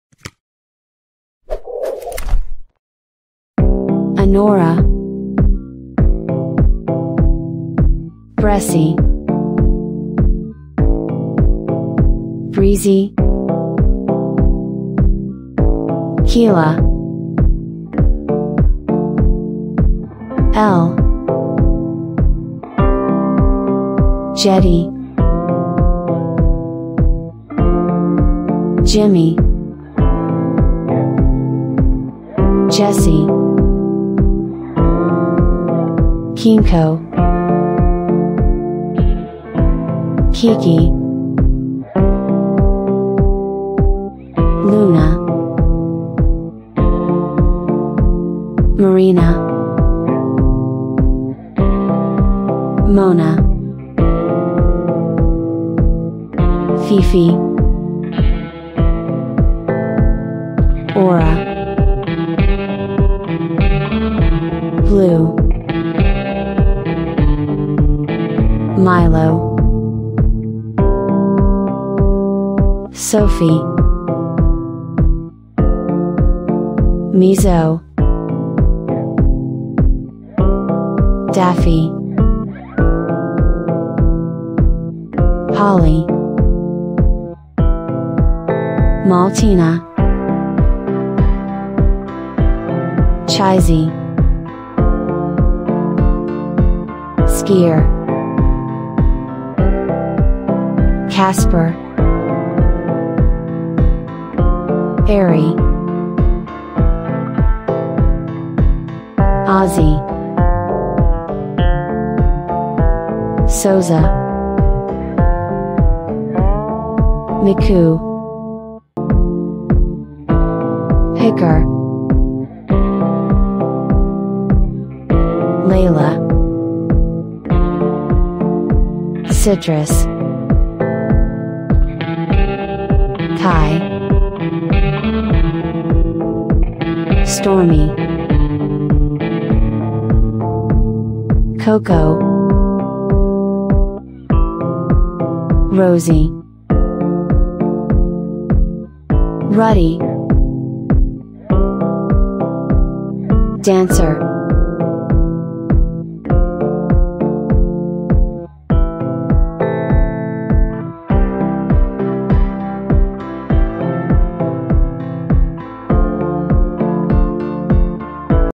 35 Beautiful Female/Girl Parrot Names - Best Birds Pet Name Ideas - Pick Your Favorite One!😉
35 Beautiful Female/Girl  Parrot Names - Best Birds Pet Name Ideas That Will Melt Your Heart!

When it comes to finding the perfect name for your female parrot, it's important to choose something that not only reflects her unique personality but also showcases her beautiful and intelligent nature. A female parrot's name should capture her distinctive qualities while being easy to pronounce and remember.

We will help you to find out the Beautiful and Modern Name For Your Pets.